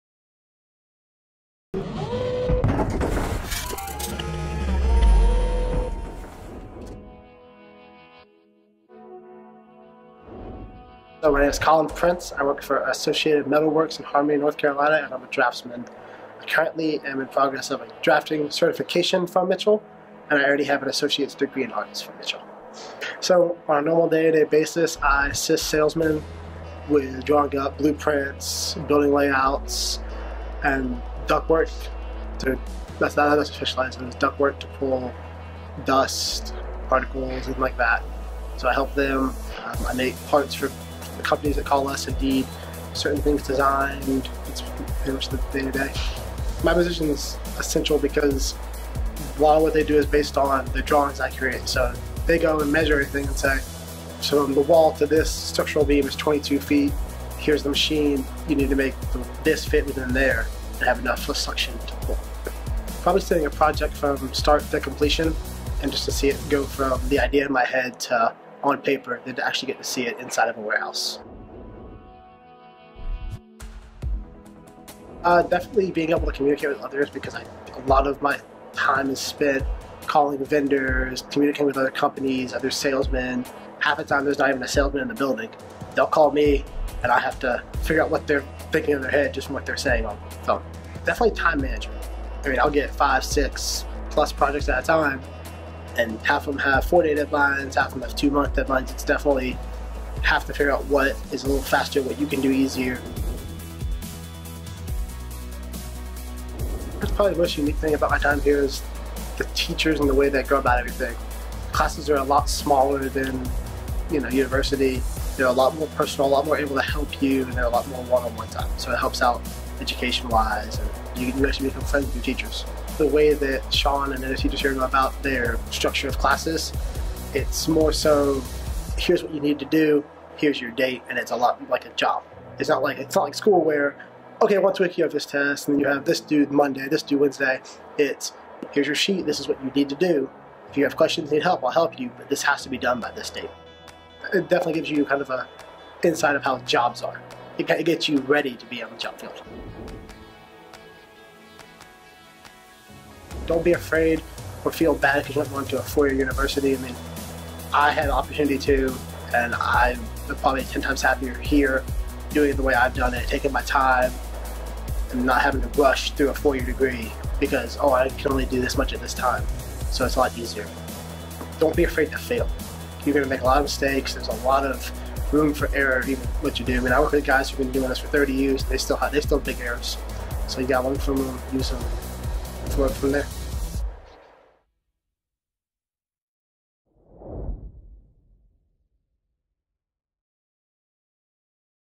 Engineering Student Colin Prince Explains Why He Chose Mitchell
Discover Engineering programs at Mitchell!

In this video, engineering student Colin Prince explains why he chose Mitchell.

Whether your goal is to enter the workforce quickly or to continue on to a 4-year university, Mitchell has a path in Engineering for you!